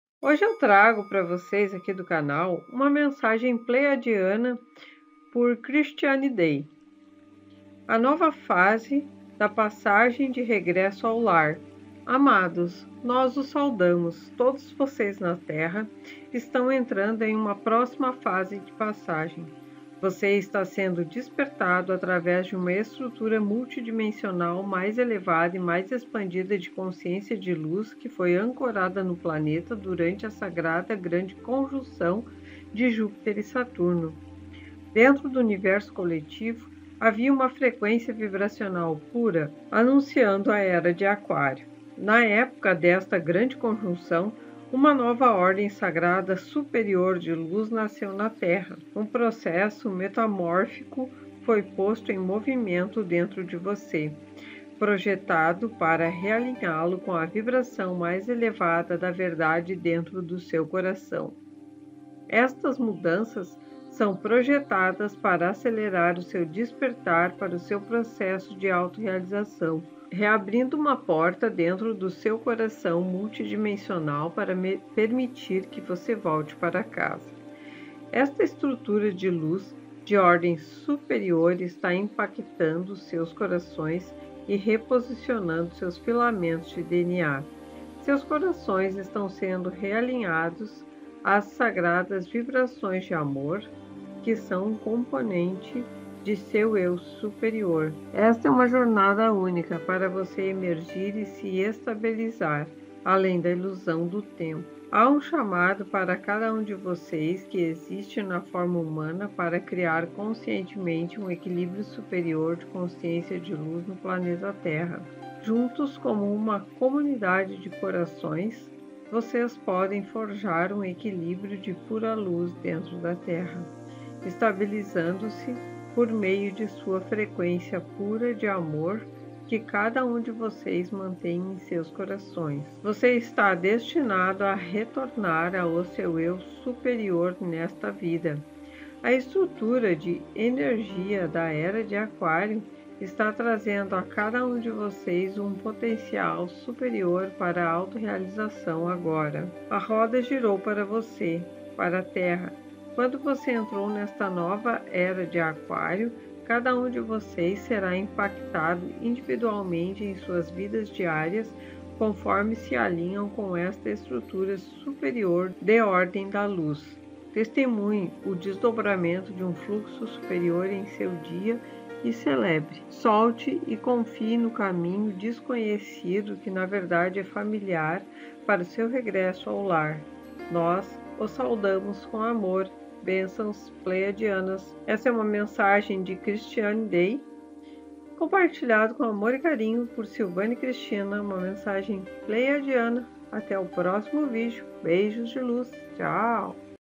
NOVA FASE DA PASSAGEM DE REGRESSO AO LAR
Mensagem Pleiadiana

Todos vocês na Terra estão entrando em uma próxima fase de passagem. Você está sendo despertado através de uma Estrutura multidimensional mais elevada e mais expandida de consciência de luz que foi ancorada no planeta durante a sagrada Grande Conjunção de Júpiter e Saturno. Dentro do Universo coletivo havia uma frequência vibracional pura, anunciando a “Era de Aquário”.

Tradução autorizada: Vilma Capuano